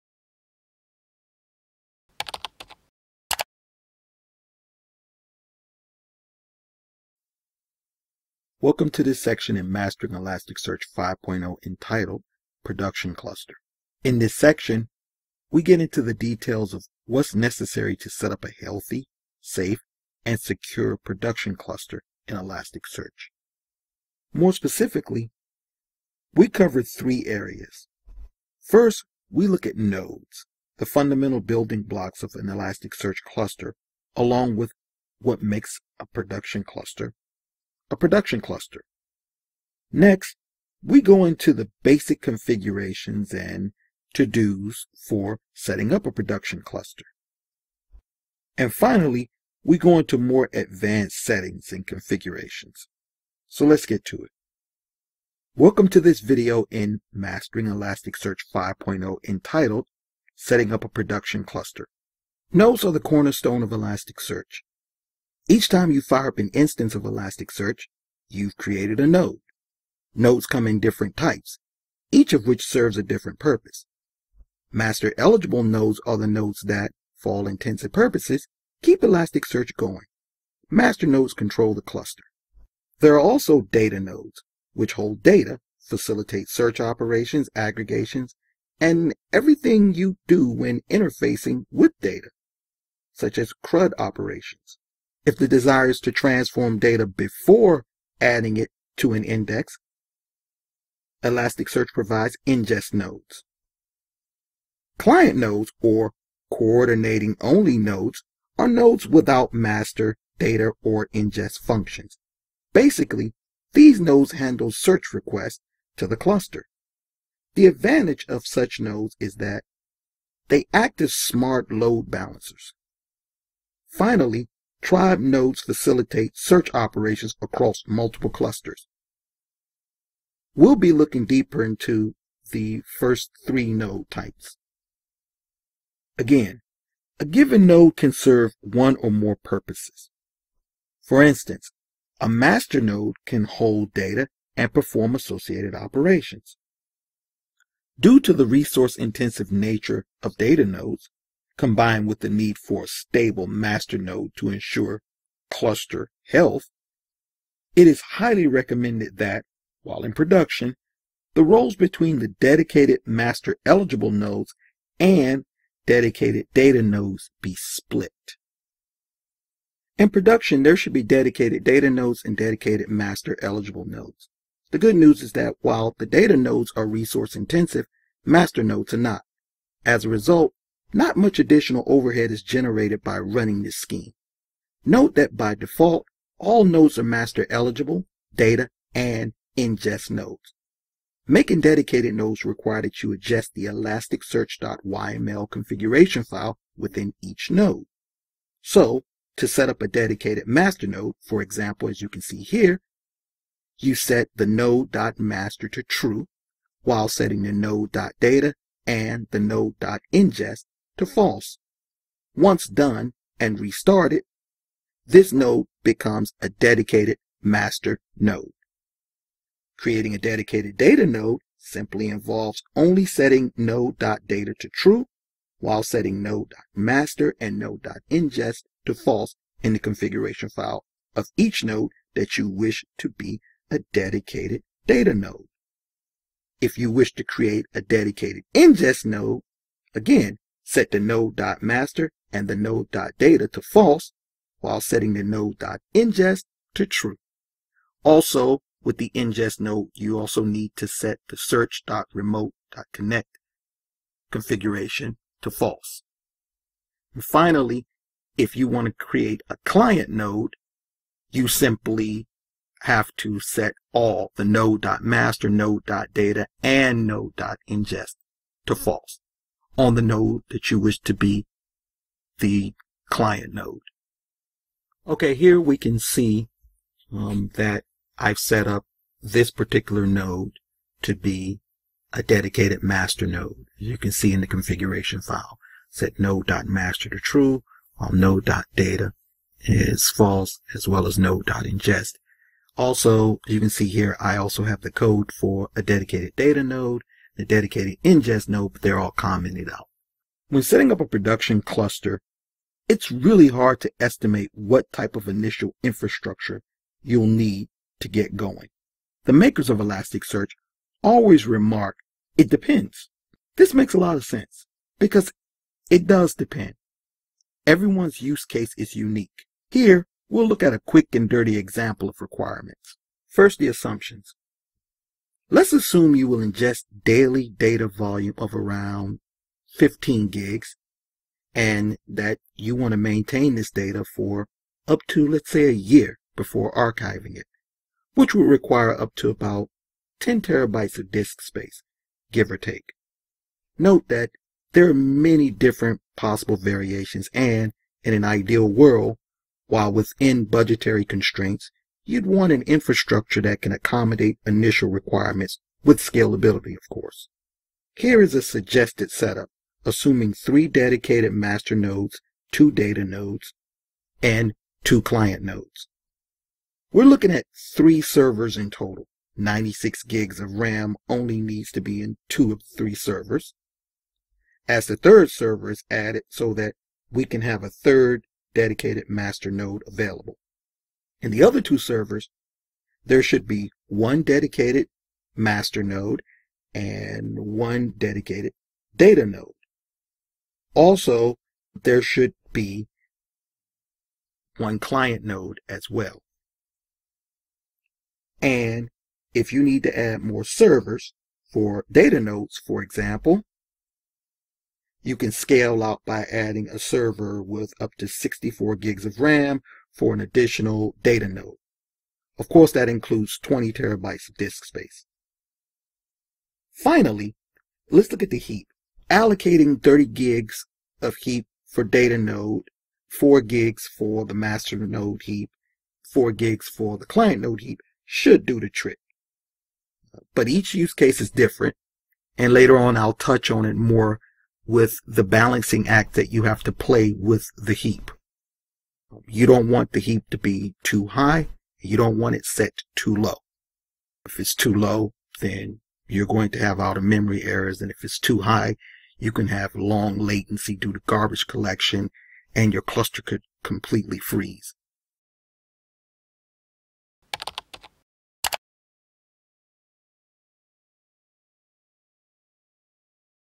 Mastering ElasticSearch : Setting Up Production Cluster | packtpub.com
Setting up a production ready cluster in ElasticSearch is the most often overlooked challenge to learning ElasticSearch. Configurations aside, understanding where to start relative to your needs can be an enormous task.
• Focus on nodes in an ElasticSearch cluster
• Learn the importance of dedicated data and master nodes
• See an example of beginning infrastructure requirements for production cluster